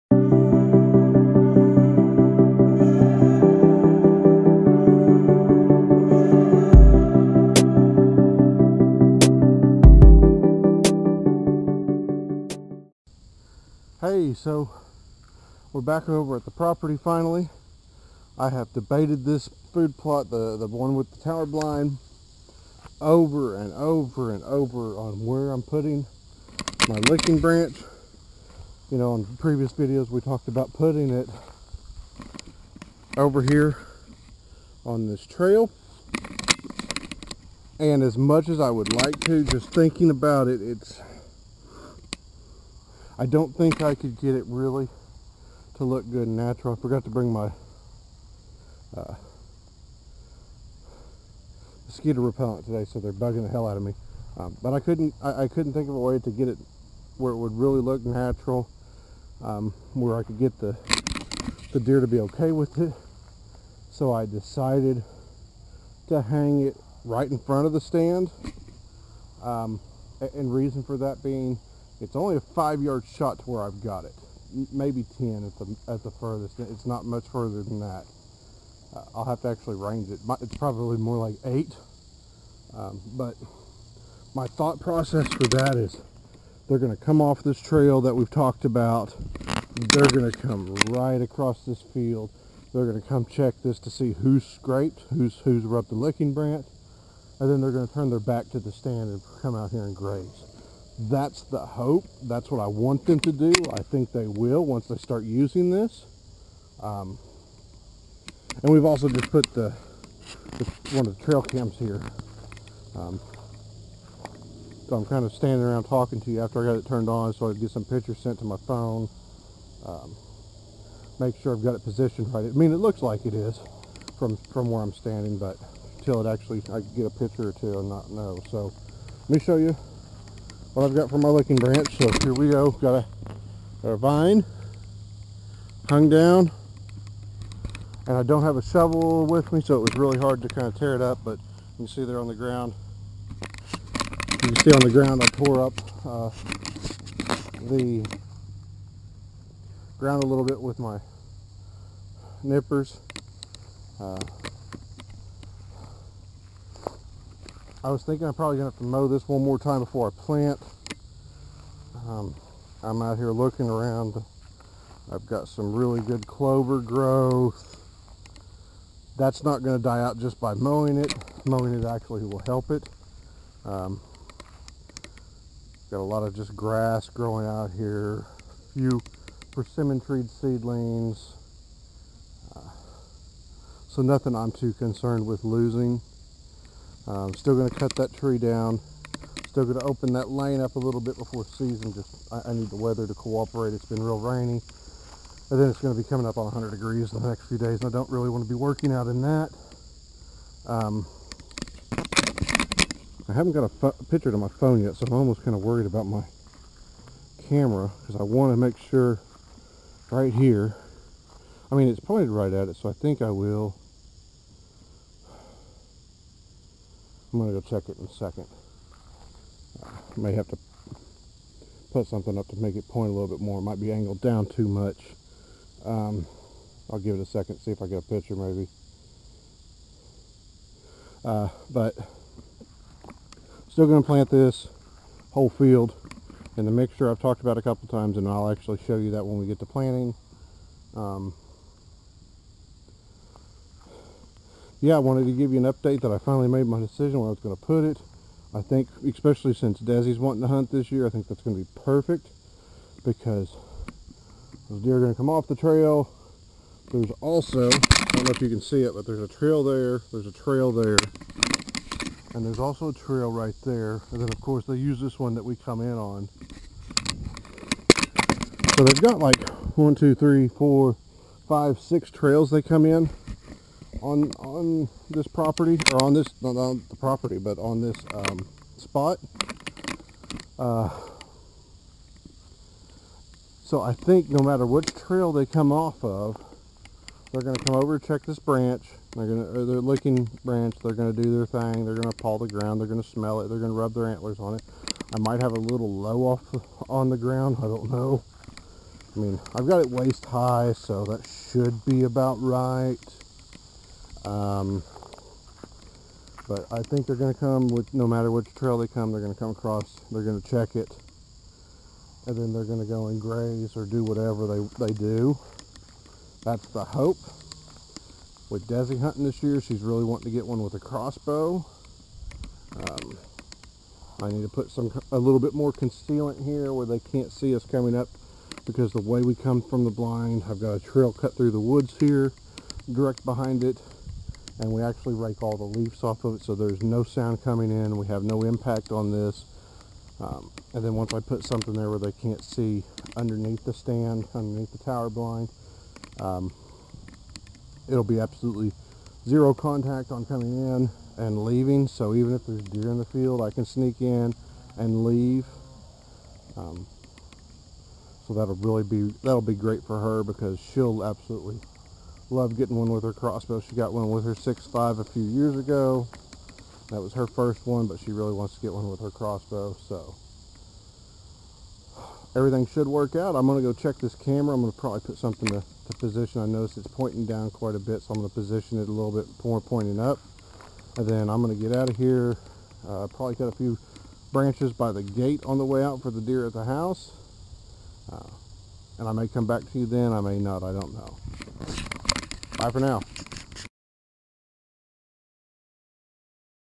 Tower Blind Licking Branch
I placed the licking branch for the tower blind stand finally. I debated this over the past couple of weeks and finally decided on a location.